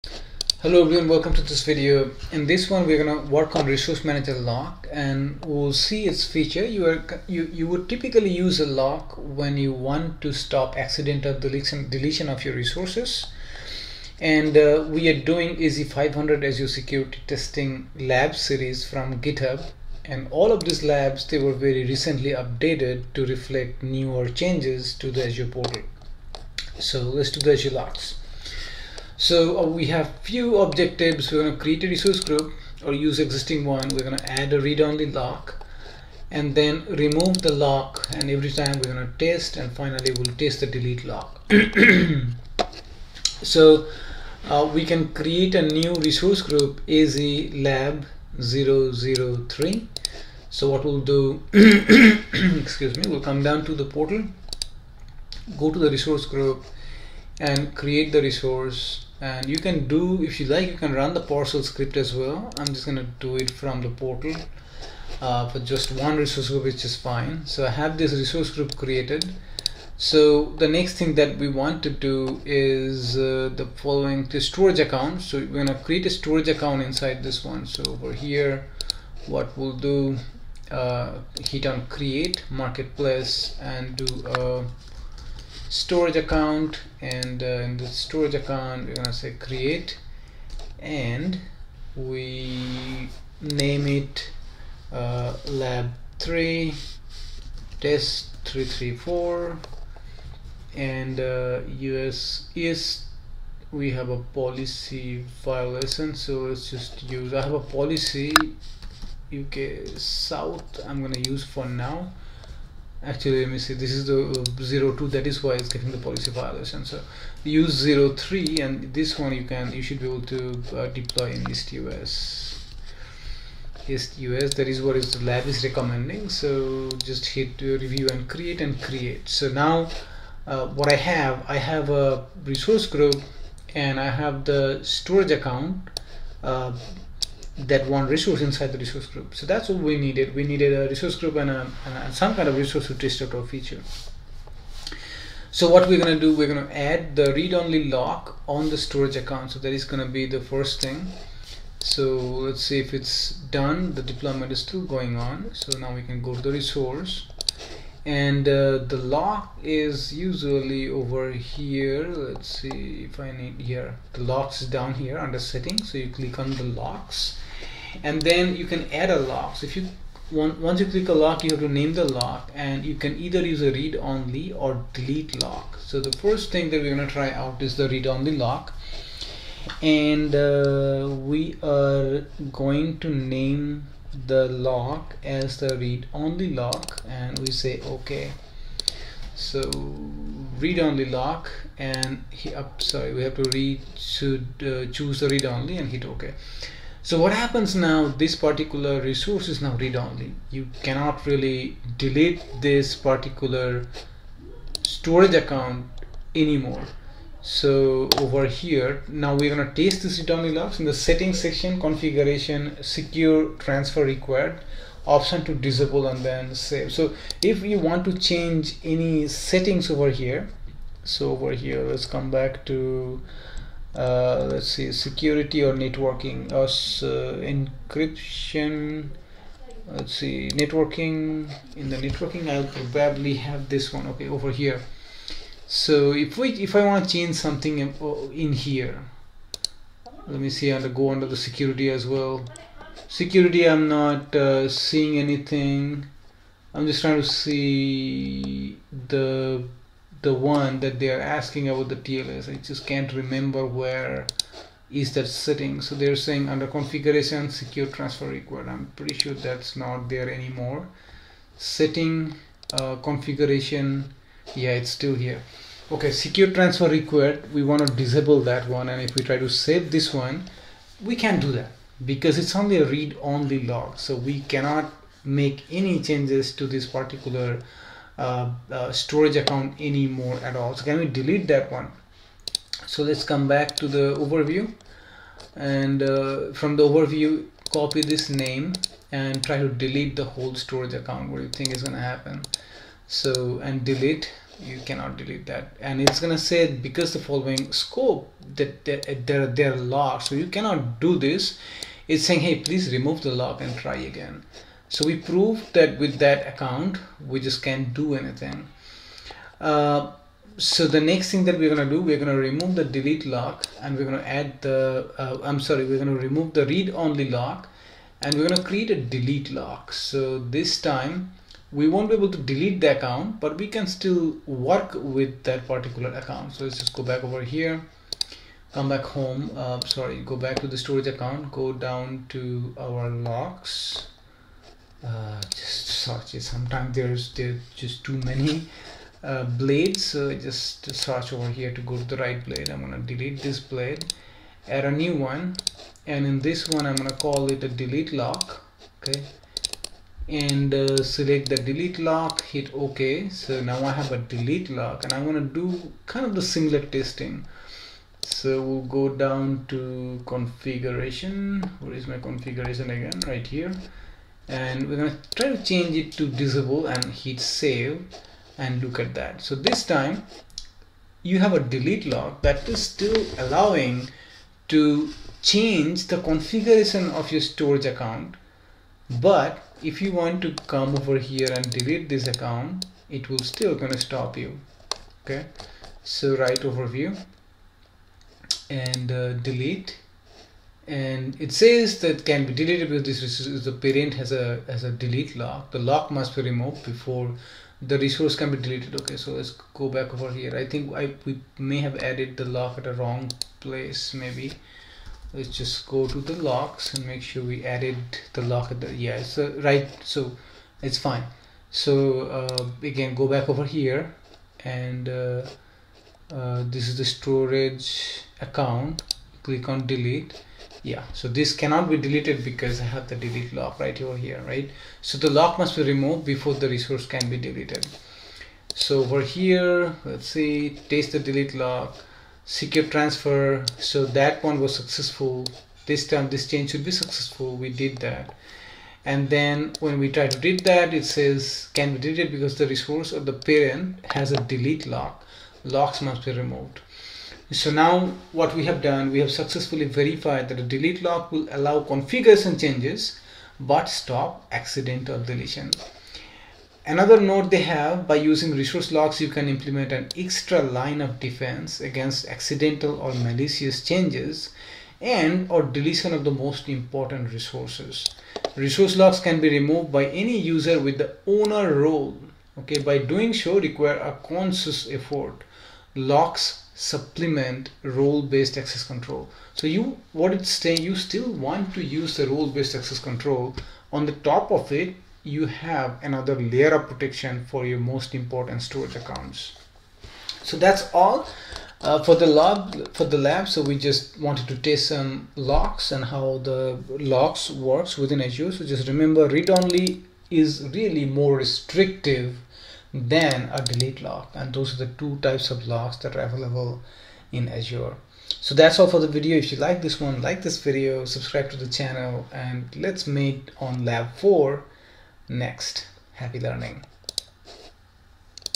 AZ 500 Azure Security Technologies Lab 03: Resource Manager Locks (updated June 2021) Hands on Lab
Tasks covered:

    Task 1: Create a resource group with a storage account.
    Task 2: Add a ReadOnly lock on the storage account.
    Task 3: Test the ReadOnly lock.
    Task 4: Remove the ReadOnly lock and create a Delete lock.
    Task 5: Test the Delete lock.